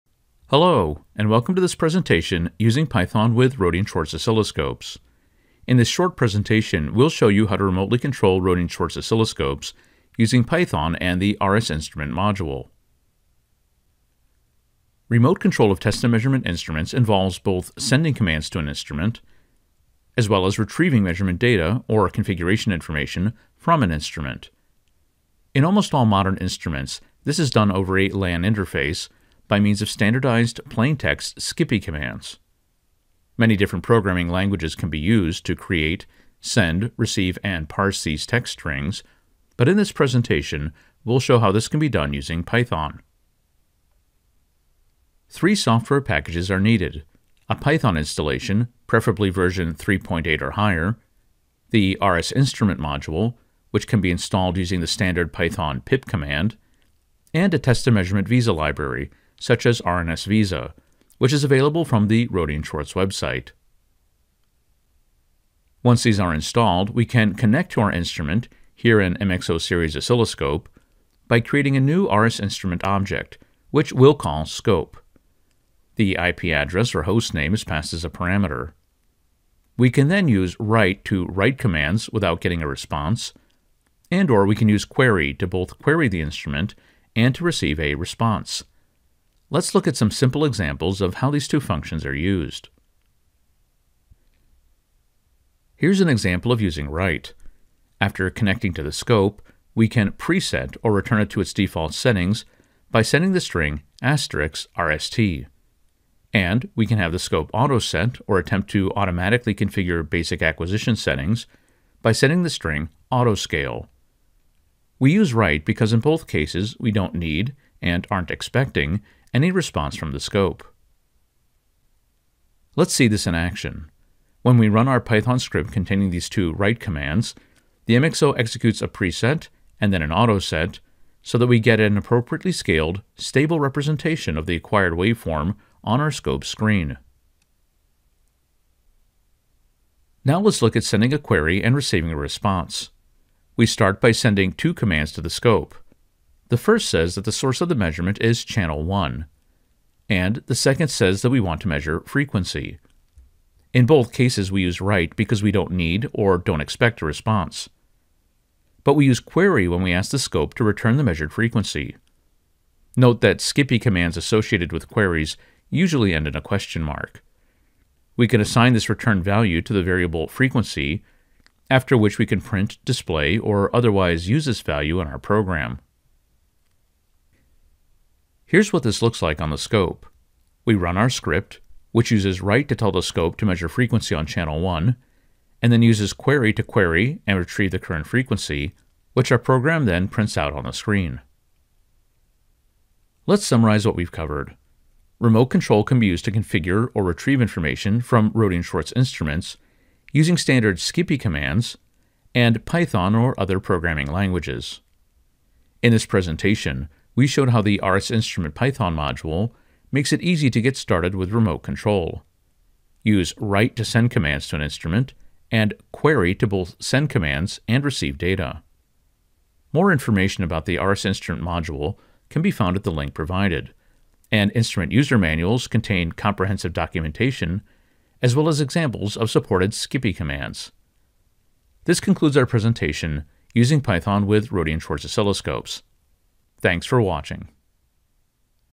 Using Python with Rohde & Schwarz Oscilloscopes
This video shows how to remotely control Rohde and Schwarz oscilloscopes using Python and the R&S Instrument module.

Timeline:
00:00 Introduction
00:13 About remote control
00:44 What you need to get started
01:04 Connecting to the instrument
01:34 Writing a command with write()
02:01 Example of write()
02:18 Sending a query with query()
02:57 Example of query()
03:12 Summary